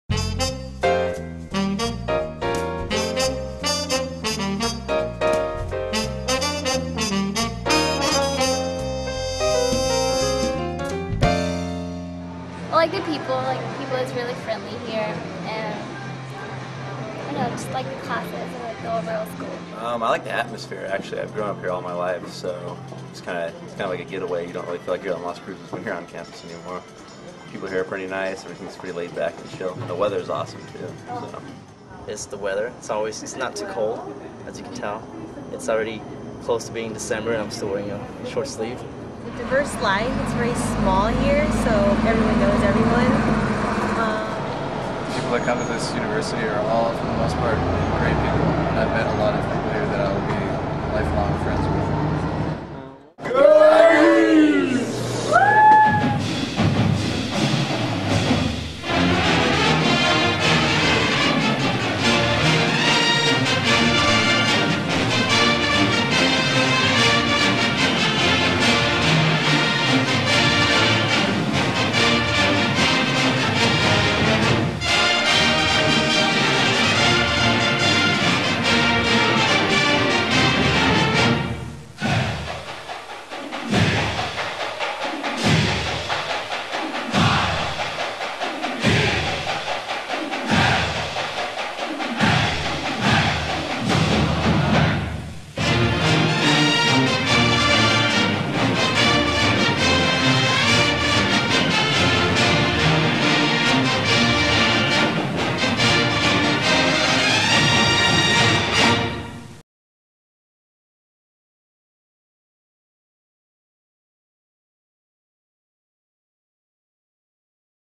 LIve, Learn, Thrive
Student video produced for "Intro to TV Production"(Journalism 302) class at NMSU.
Instructor: Roger P. Mellen

This video shows what is great about
NMSU, with music supplied by the pep band.
This video was made by Brock Haroldson and Richard Romero for JOUR 302